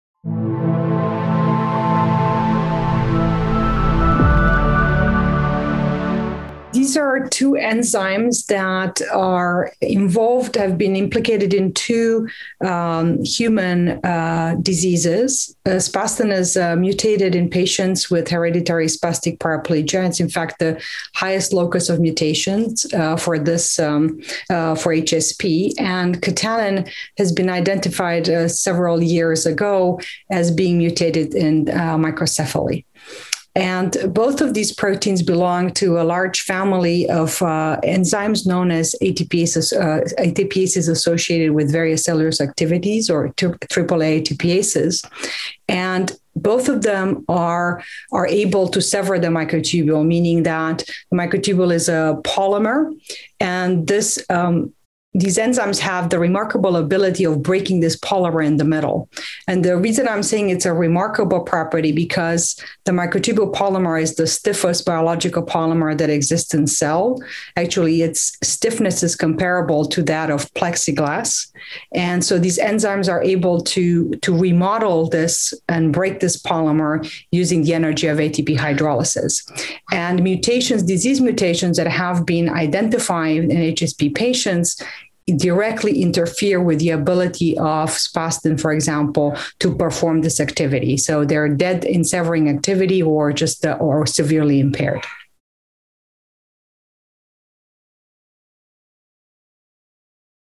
The roles of spastin and katanin in microtubule severing
Antonina Roll-Mecak, PhD, Unit of Cell Biology and Biophysics, National Institute of Neurological Disorders and Stroke, Bethesda, MD, talks on spastin and katanin, microtubule-severing enzymes implicated in several rare neurological conditions. Both enzymes are ATPases Associated with diverse cellular Activities (AAA ATPases), which can severe microtubules along their length by ATP hydrolysis. Mutations in spastin and katanin severely impair their severing ability, leading to fewer and more disorganized microtubules. This interview took place during the American Academy of Neurology (AAN) 2021 Annual Meeting.